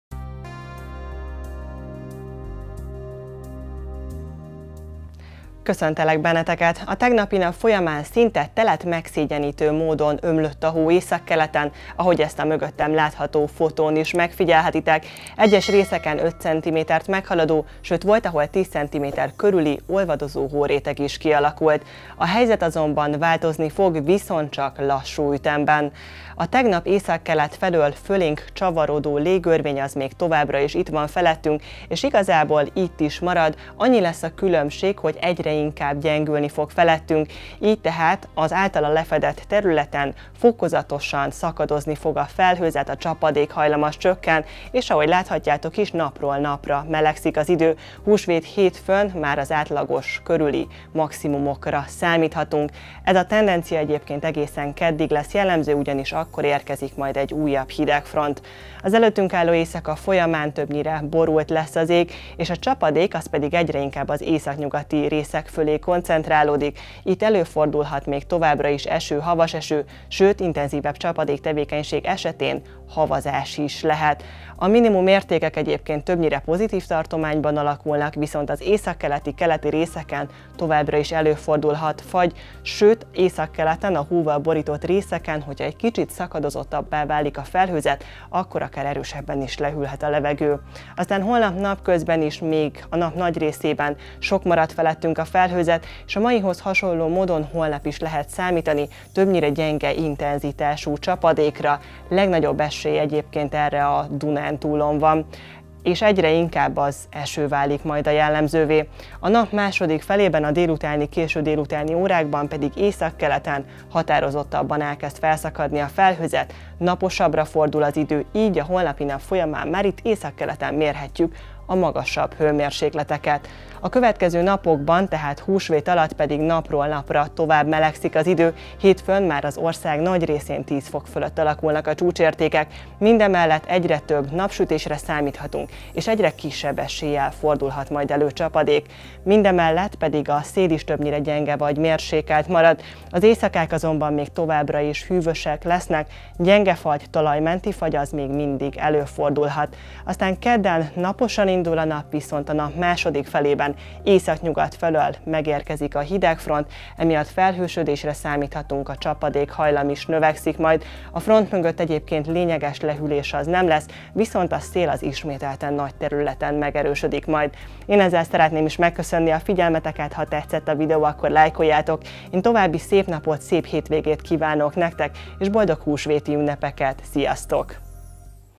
Lassan...
...enyhül az idő, szűnik a csapadék.
Húsvétra egyre jobban szakadozik a felhőzet, így hétfőn már a napsütés lesz a túlsúlyban, és ekkor már csapadéknak is kicsi az esélye. A maximumok ugyan emelkednek, ám éjszaka gyenge fagy, talajmenti fagy továbbra is előfordulhat.

Ha érdekel az időjárás, akkor kövess be minket az Országos Meteorológiai Szolgálat közösségi média felületein: